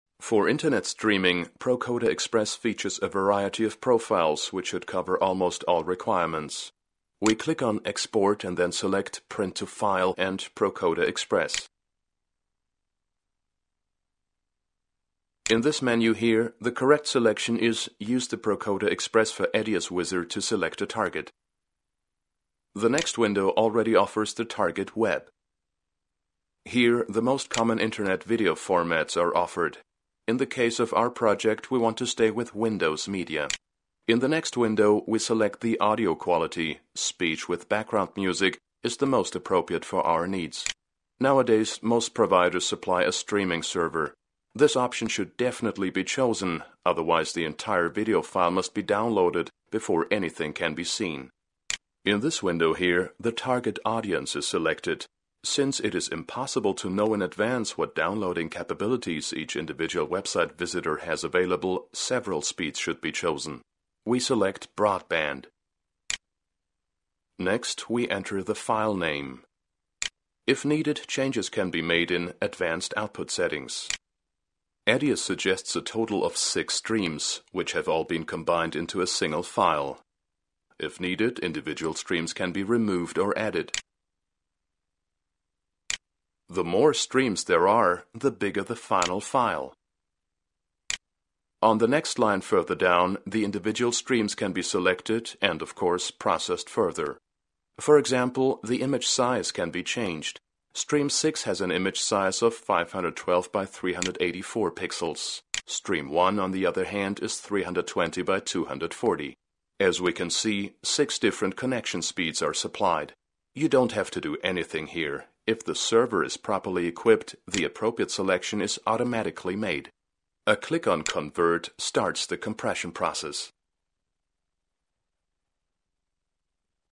08.03. Output for Internet Streaming [EDIUS 4]
How to prepare video for Web Streaming via ProCoder Express for EDIUS..

A tutorial from the series of 'getting-started' videos for the older 4th version of EDIUS. Despite this fact and their rather poor video quality,
an EDIUS beginner might still find some of the topics to be of interest.